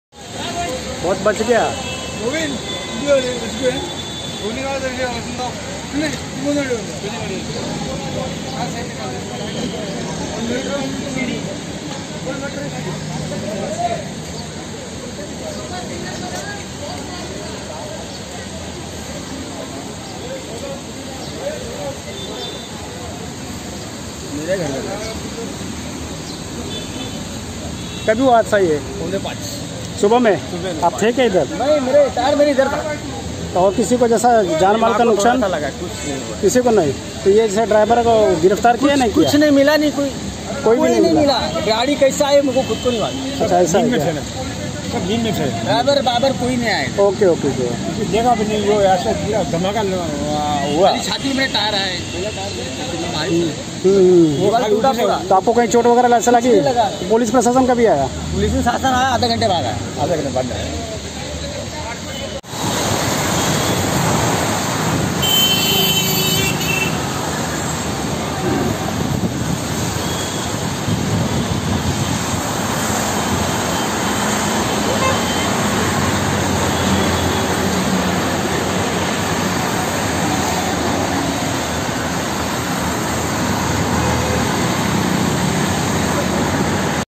🚚🚧Trailer Truck Highway se Barricade Toddkar Service Road per Gira.
🚧 🛣️ 🚚 *Jogeshwari Me Trailer Truck Bridge Barricade Todkar Niche Gira.*

*🚚🛣️🚧 Aaj subha Jogeshwari East Westren Express Highway Par Tezz Raftar Out Of Control Ek Trailer Truck Birgde Ka Barricade Todkar Niche Service Road Per Gira.*

*🚚🏃‍♂️Iss Ghatna Ke Baad Trailer Truck Driver Spot Se Farar.*

*🚨👨‍✈️🚒👷‍♂️Police Ke Sath Fire Brigade Ki Team Mauke Per Pahochi.*